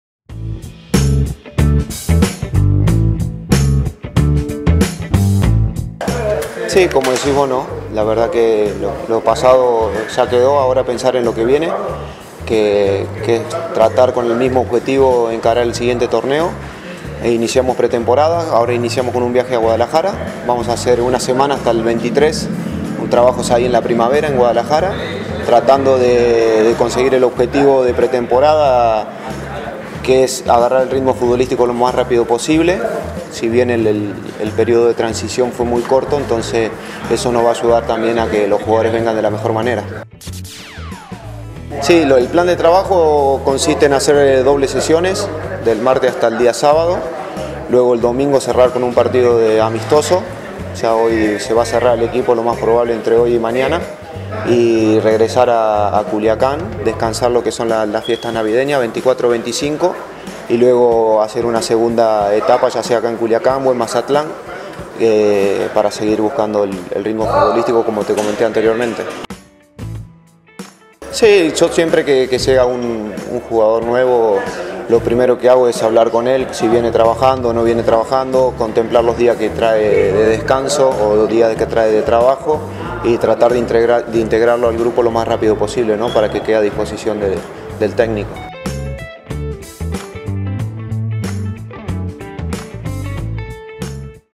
José Altieri sobre la Pretemporada
Nuestro Preparador Físico habló sobre los trabajos que realizará el grupo en Jalisco.